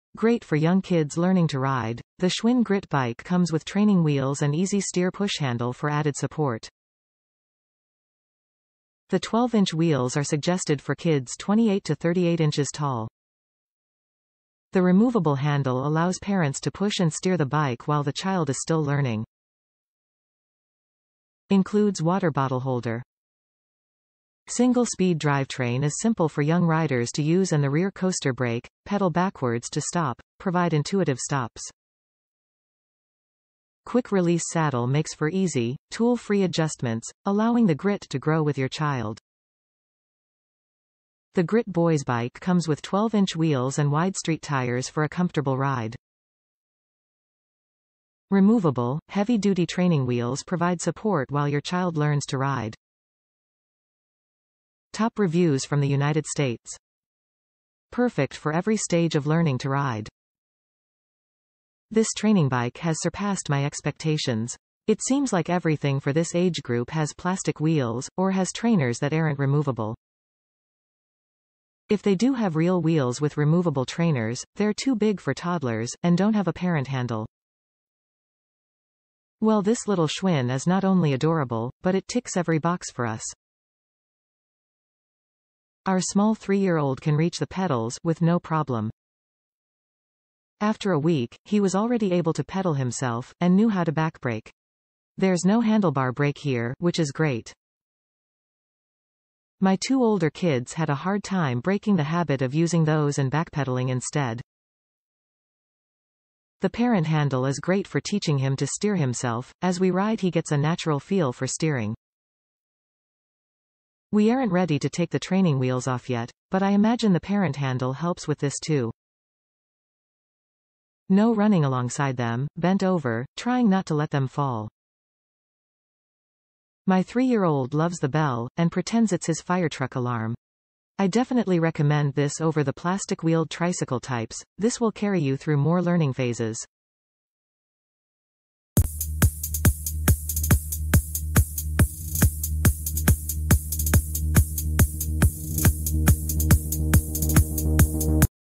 Schwinn Grit and Petunia Steerable Kids Bike, Boys and Girls Beginner Bicycle, 12-Inch Wheels
Great for young kids learning to ride, the Schwinn Grit bike comes with training wheels and easy-steer push handle for added support. The 12-inch wheels are suggested for kids 28 to 38 inches tall.
The removable handle allows parents to push and steer the bike while the child is still learning. Includes water bottle holder.
Single speed drivetrain is simple for young riders to use and the rear coaster brake (pedal backwards to stop) provide intuitive stops.
Quick-release saddle makes for easy, tool-free adjustments, allowing the Grit to grow with your child
The Grit boy's bike comes with 12 inch wheels and wide street tires for a comfortable ride. Removable, heavy duty training wheels provide support while your child learns to ride.

Perfect for every stage of learning to ride
This training bike has surpassed my expectations! It seems like everything for this age group has plastic wheels, or has trainers that aren't removable. If they do have real wheels with removable trainers, they're too big for toddlers, and don't have a parent handle. Well this little Schwinn is not only adorable, but it ticks every box for us. Our small 3 year old can reach the pedals, with no problem. After a week, he was already able to pedal himself, and knew how to backbreak! There's no handlebar break here, which is great. My two older kids had a hard time breaking the habit of using those and backpedaling instead. The parent handle is great for teaching him to steer himself, as we ride he gets a natural feel for steering. We aren't ready to take the training wheels off yet, but I imagine the parent handle helps with this too. No running alongside them, bent over, trying not to let them fall. My 3 year old loves the bell, and pretends it's his fire truck alarm! I definitely recommend this over the plastic wheeled tricycle types, this will carry you through more learning phases.